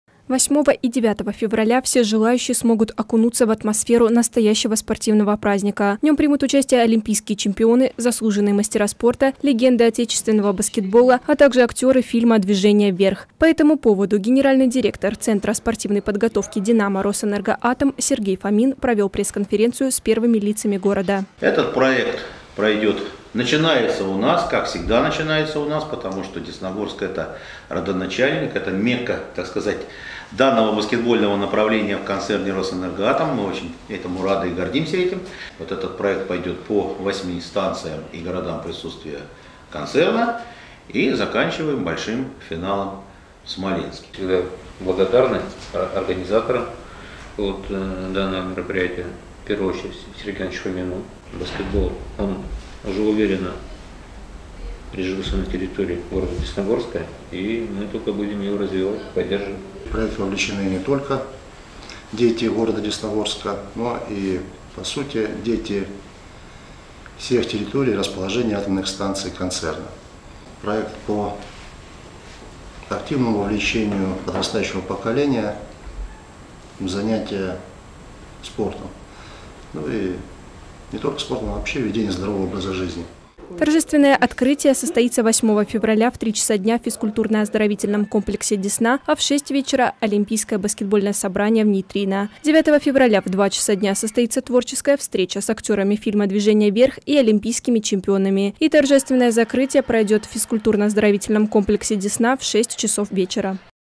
Десна-ТВ: В Десногорске стартуют олимпийские дни баскетбола
В Десногорске пройдёт большое событие -  Администрация Смоленской области, концерн «Росэнергоатом», школьная баскетбольная лига «КЭС-баскет» при поддержке центра спортивной подготовки «Динамо-Росэнергоатом» и Федерации баскетбола Смоленской области проводит олимпийские дни баскетбола.